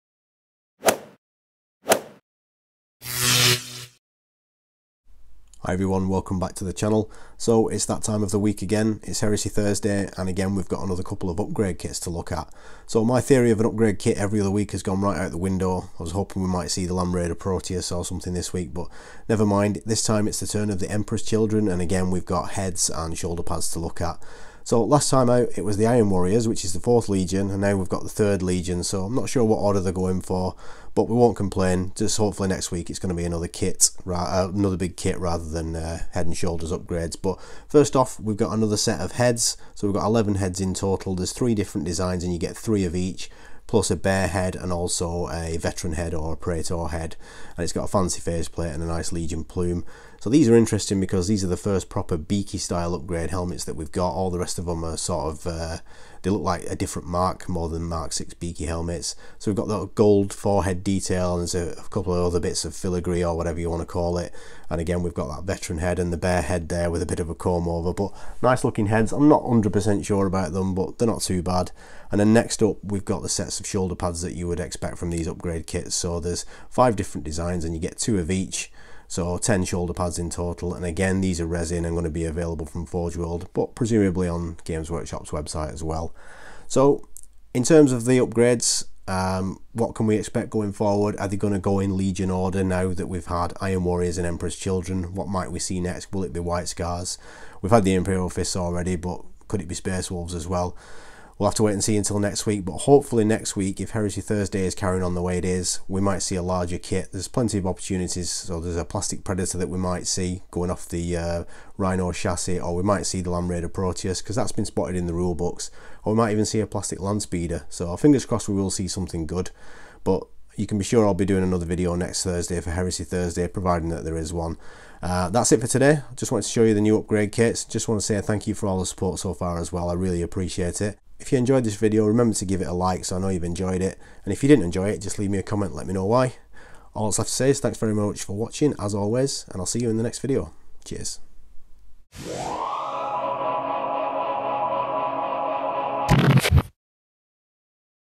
NEW Emperor's Children UPGRADES for Horus Heresy!
It's that time of the week - it's Heresy Thursday again and this week its new upgrade packs for the perfectionists out there, The Emperor's Children.

Well it appears my theory of an upgrade pack every other week  has gone out the window, but hopefully next week we might see another plastic kit revealed, and going off the leaked images that have been floating around tonight, my guess is plastic Predator!

As usual for these upgrade packs, we get one with 11 heads comprising of 3 different helmets of which you get 3 each, plus a Sergeant/Officer helmet and an unhelmeted head, and the other pack contains 10 shoulder pads featuring the Emperor's Children icon - there's 5 designs and you get 2 of each. The helmets are interesting as they are proper beakie style helmets, with added EC decoration, and the Sergeant/Praetor helmet is particularly nice with the faceplate and plume.

I'd say these are a good fit for the III legion and I like how they've gone with  Beaky helmets rather than all having faceplates like the sgt/veteran/praetor one. The shoulder pads are very nice too, probably my favourites so far.

Hopefully for Heresy Thursday next week we'll be getting another plastic kit reveal, either the predator or maybe the Land Raider Proteus - watch this space...

If you enjoyed this video then please let me know by liking it.

Thanks ever so much for watching and for your continued support, it really means a lot!